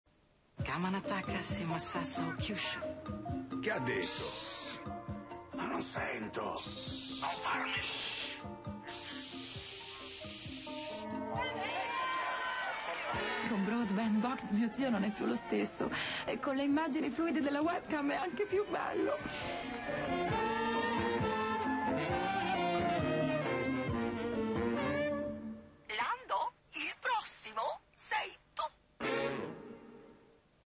Telecom e le offerte del passato.Sembrano passati 100 anni!!
L'offerta Telecom Italia della linea veloce. Erano gli anni 2000, sembrano però passati tantissimi anni!